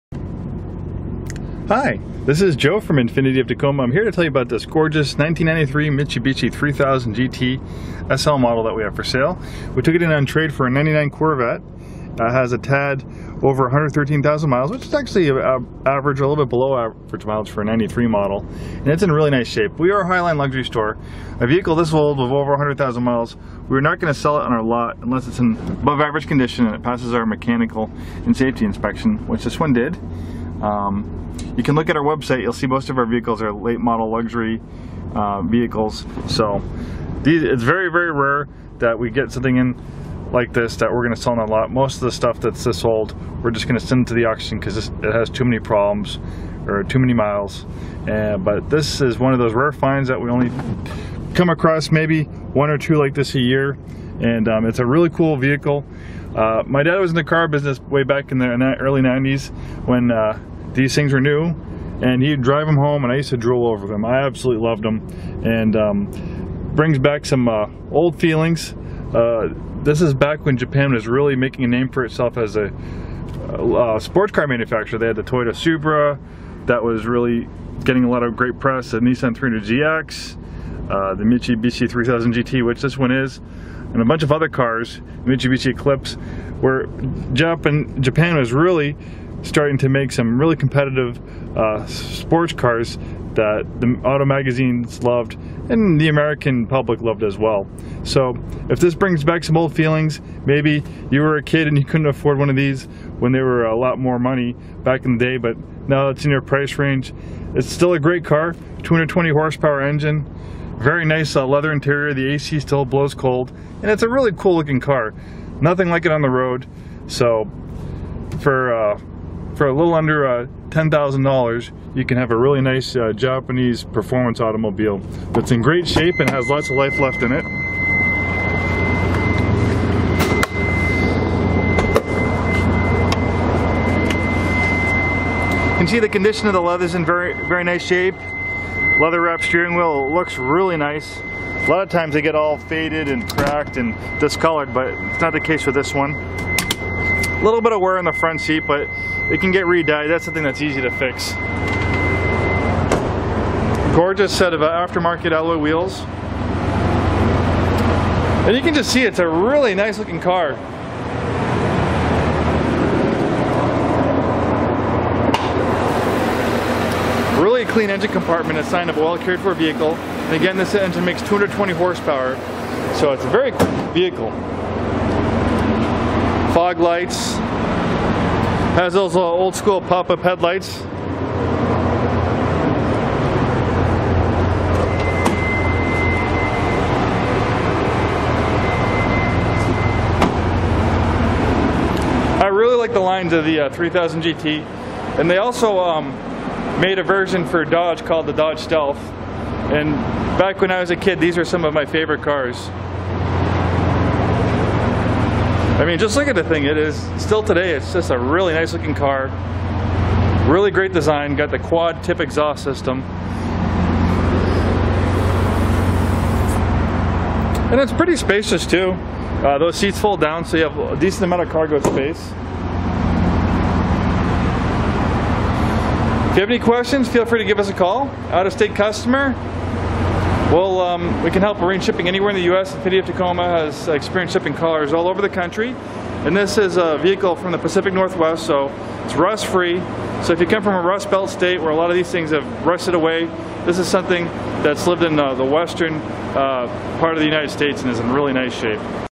1993 Mitsubishi 3000GT 3560A
1993 MITSUBISHI 3000GT Fife, WA
Stock# 3560A

Clean Carfax vehicle from the Pacific Northwest, runs and drives great, rare find, Japanese classic sports car!! The early nineties were the heyday of Japanese sports car and the 3000GT was one of the coolest ones around. With gorgeous lines, outstanding build quality and a luxury filled interior its very comfortable as a daily driver and economical to own. The V6 makes 220-hp, plenty of power to backup this 3000GTs performance looks. We took this one in on trade for a Corvette and it was in such great shape we decided to sell it at our store. In most cases older cars like this we do not sell on our lot, but since this one is in such great shape, physically and mechanically, we decided to make an exception. Sale prices shown do not include sales tax and license. A negotiable Documentary service fee, up to $150 may be added to the sale price.